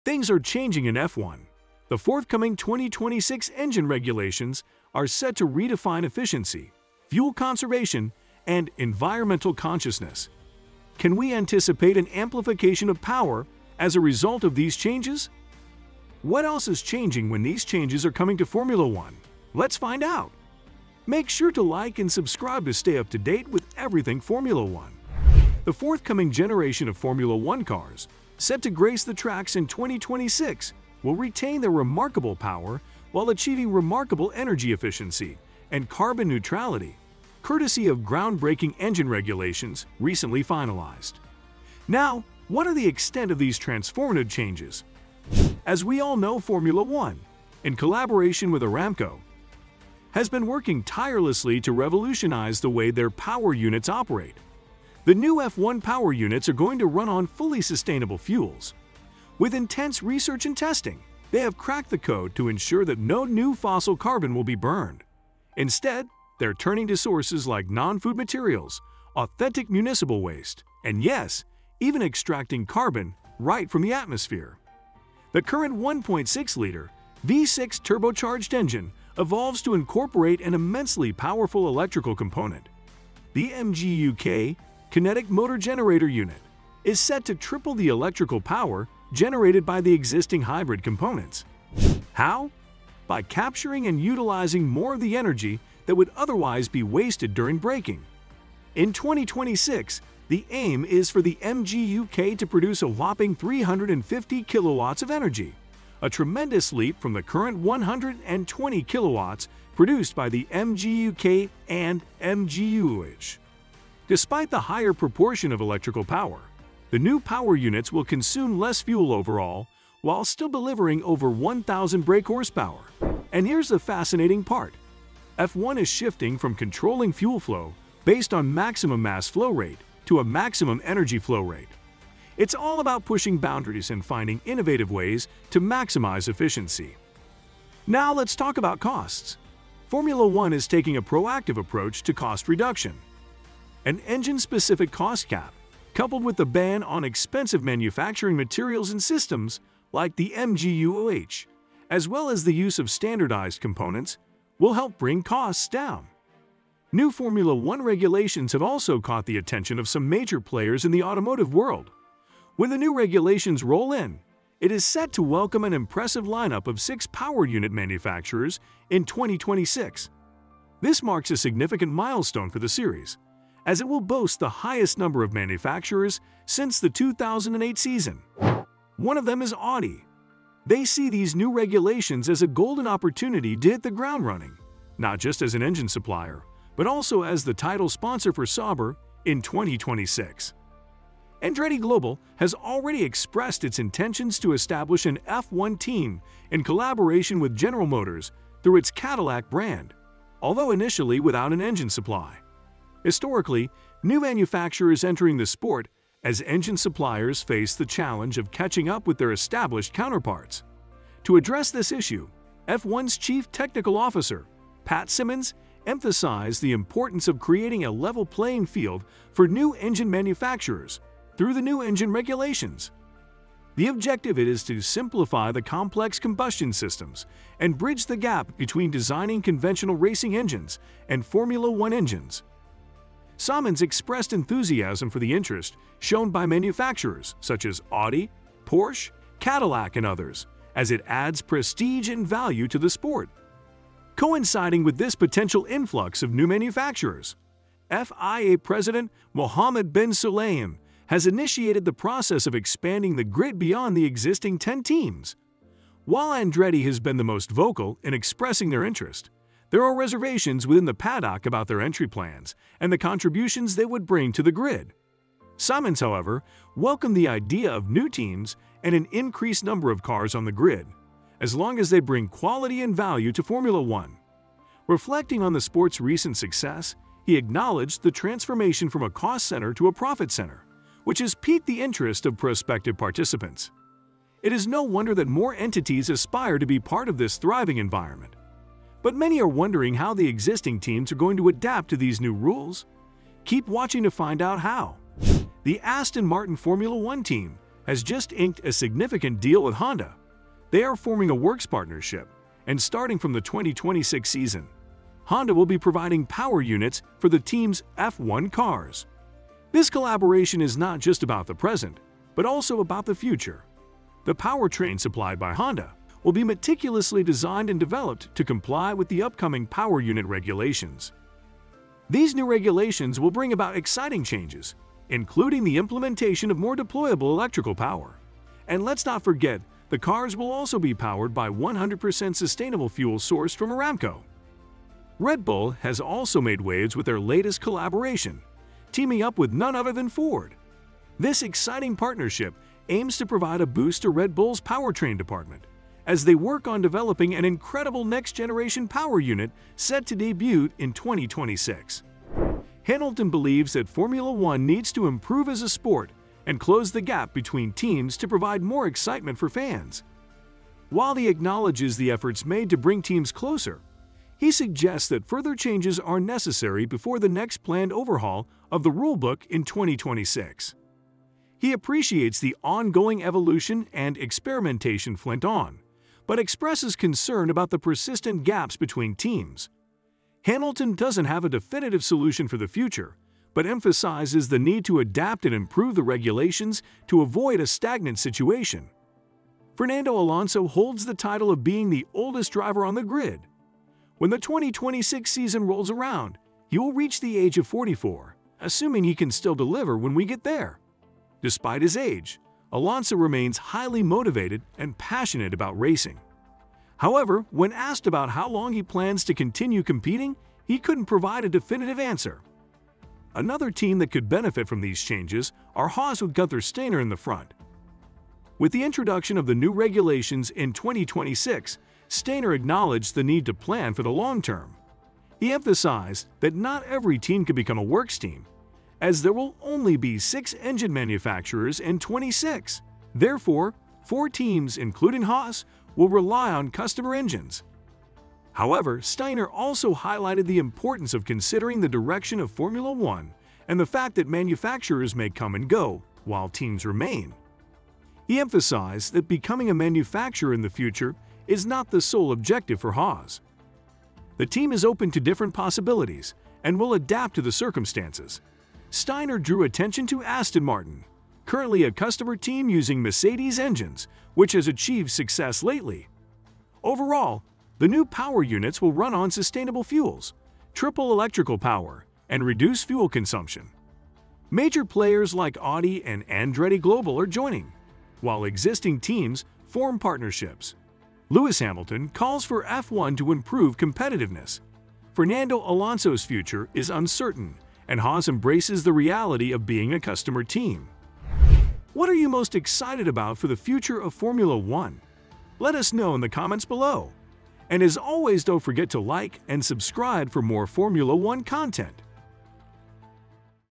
EVERYTHING You NEED To Know About F1 2026 | UPDATED
We are almost halfway through the 2023 F1 season and there are already talks about the changing scenery for the 2026 F1 season. We have compiled everything you need to know about these changes. From updates in the manufacturer part, insights into the motor and aerodynamics to the overall changes for the teams including Red Bull, Aston Martin, Haas and all the others as well. But will these updates be everything that is changing or is it to early to say? And do we think that these upgrades and changes are for the better of the sport? Let’s find out!